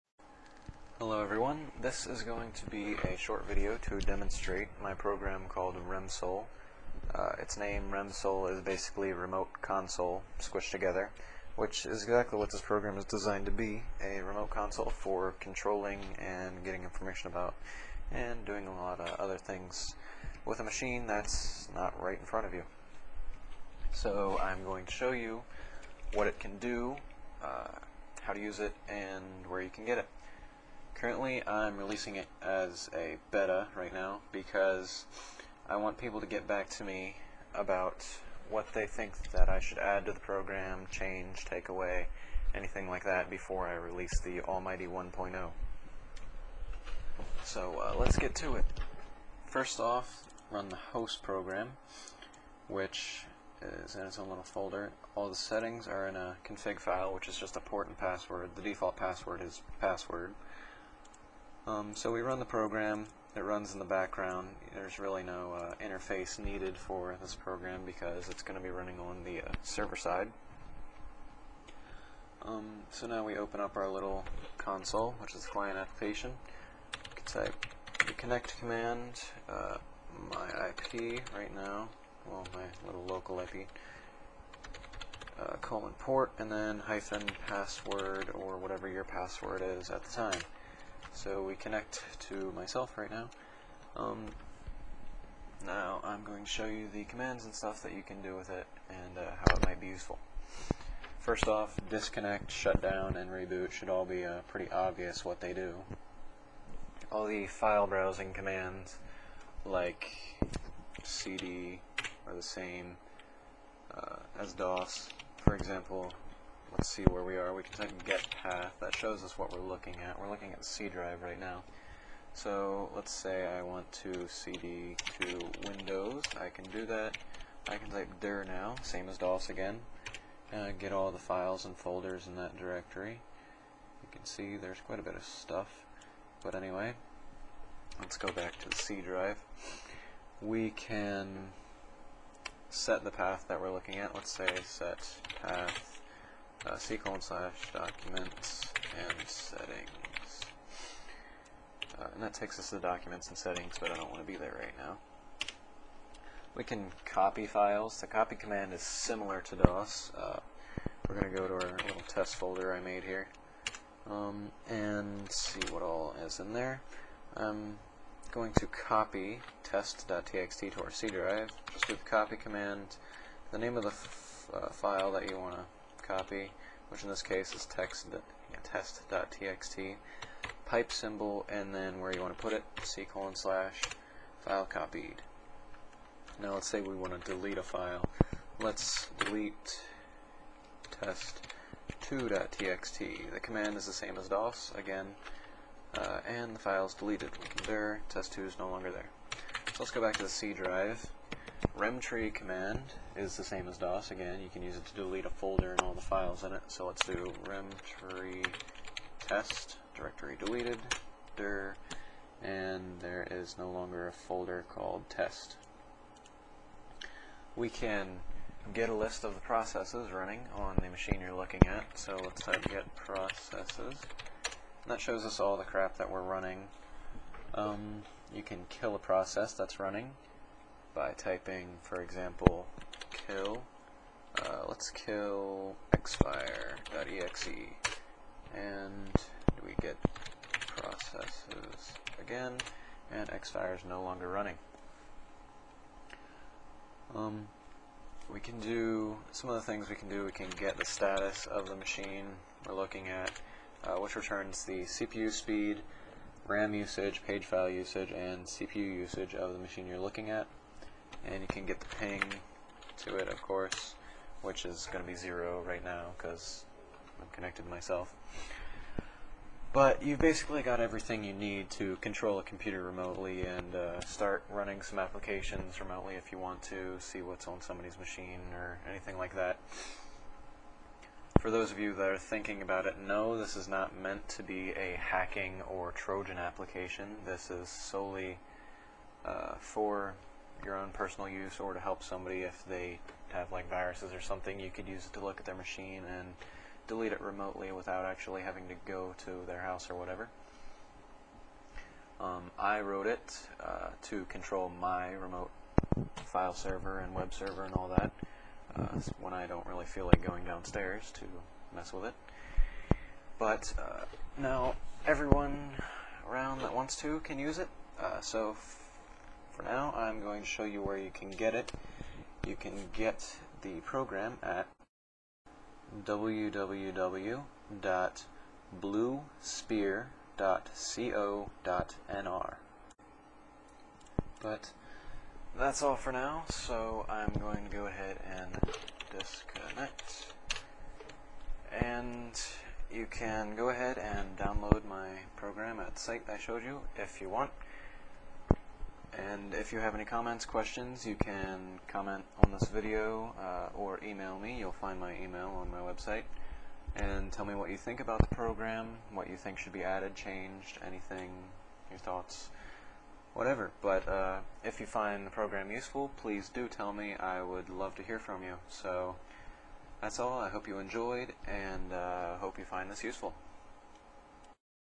RemSole - My Remote Console Program
RemSole is a remote console application that I wrote. This video is a short demonstration about it.

You can download RemSole as well as the source code